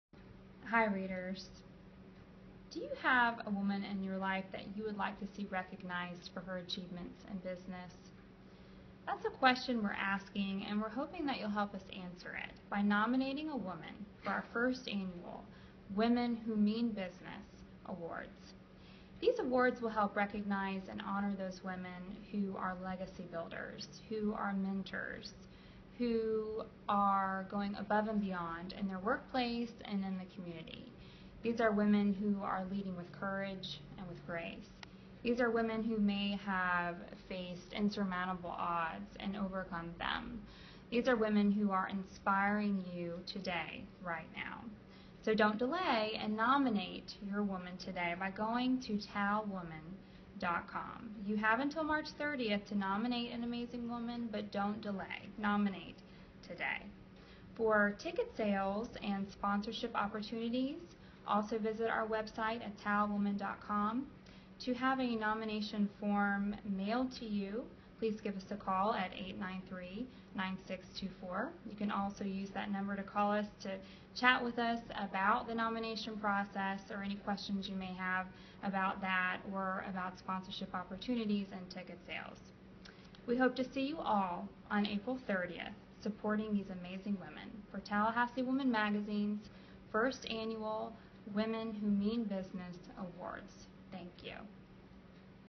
Women Who Mean Business Nominations
Heather Thomas invites you to nominate a remarkable woman in the business world. Find out more at Talwoman.com.